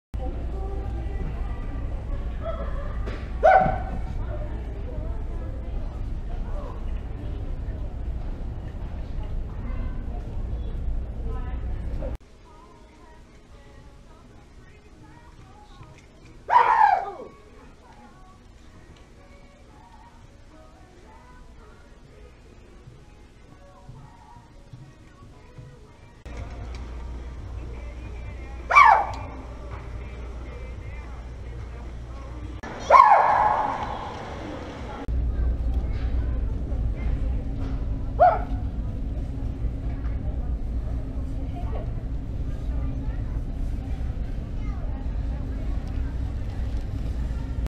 Old man Screaming Prank
I went around with this wonderful mask screaming in the stores. It was fun.  Hopefully gets you a smile.